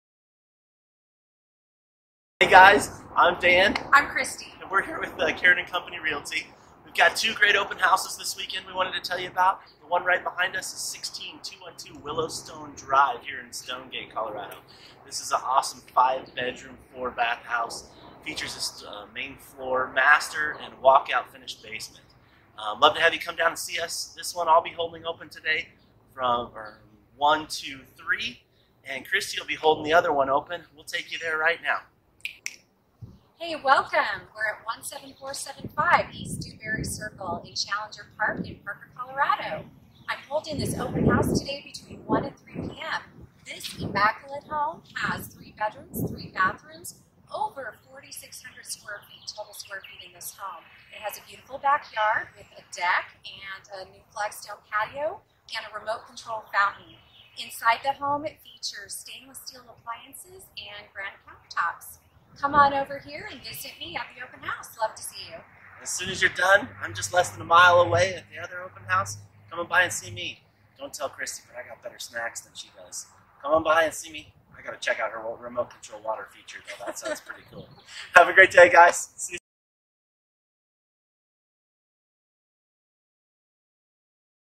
Two Open Houses on May 6 in Parker, CO
Check out the two Open Houses we have going on this weekend in Parker CO!  One is in Stonegate and one is in Challenger Park Estates!